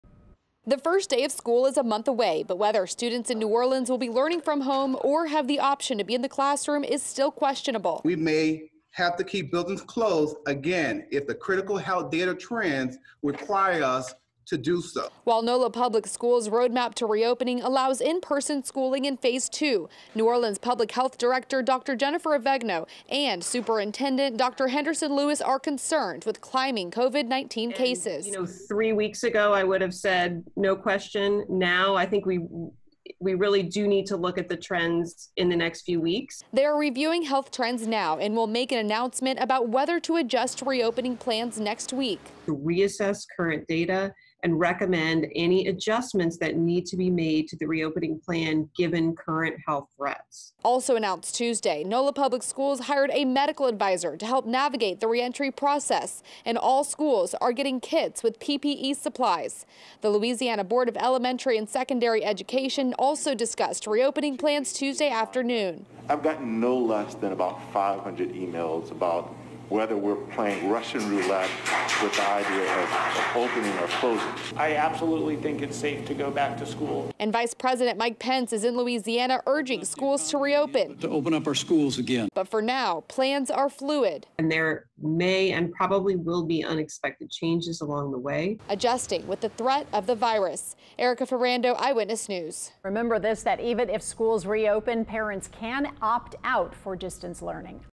New Orleans schools making decision on whether or not to reopen
New Orleans public schools have plans to reopen in about a month, but with cases rising, officials are taking a second look.